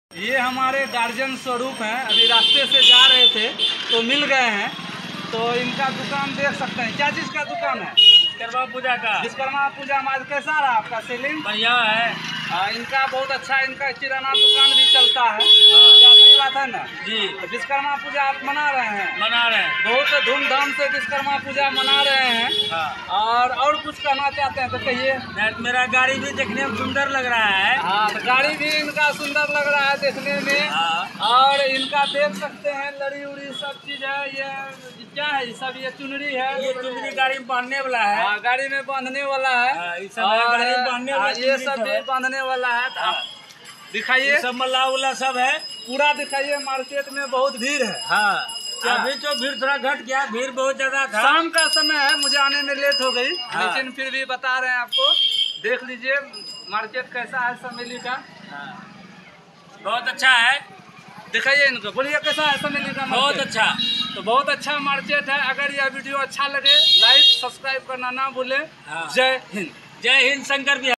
sameli khaira विश्वकर्मा पूजा बड़े धूमधाम से मनाया गया बड़े हर्षोल्लास के साथ
विश्कर्मा पूजा बड़े हर्षोल्लास के साथ मनाया गया